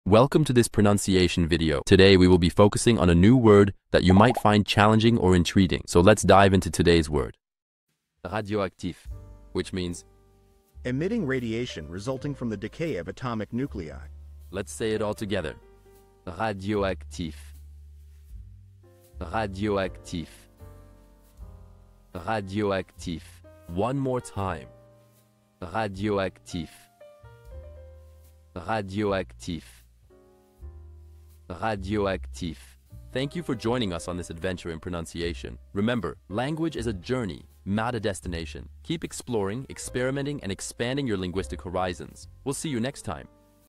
How to pronounce Radioactif
Master the Pronunciation of 'Radioactif Which means radioactive' - which means : Emitting Radiation Resulting from the Decay of Atomic Nuclei. 📝 with @BaguettePronunciation 🎓🗣️ - [Your Guide to French]

Learn to pronounce 'Radioactif' perfectly in French with our easy and engaging tutorial by @BaguettePronunciation! 📖📽️ In this comprehensive video, we provide you with native speaker examples 🗣️👂 and phonetic breakdowns 🎵 to ensure you get the pronunciation just right.

How to say Radioactive in French